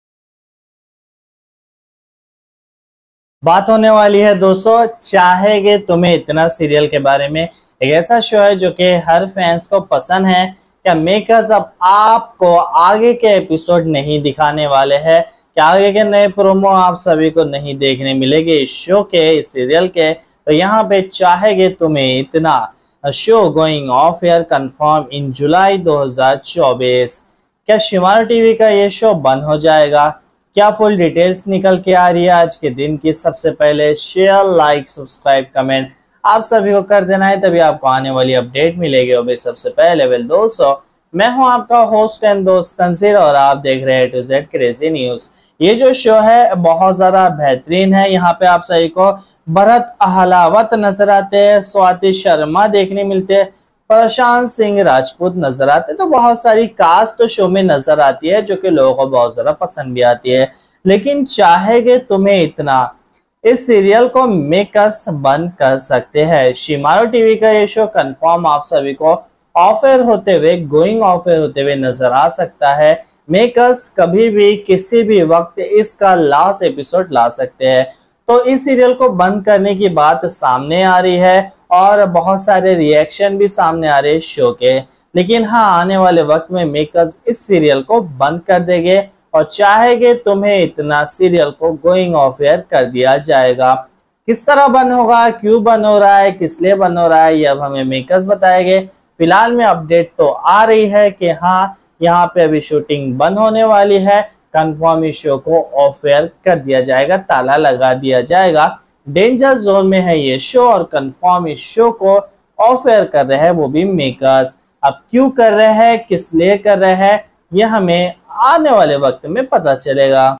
Chaahenge Tumhe Itnaa show : going off air confirm in July 2024
About this video- Dosto Aaj Ki Is Video Me Maine Aapko Ye Bataya Hai Ki Chaahenge Tumhe Itnaa show : going off air confirm in July 2024 Ummeed Hai Aapko Ye Video Bahut Hi Jyada Pasand Aayegi Thank You So Much.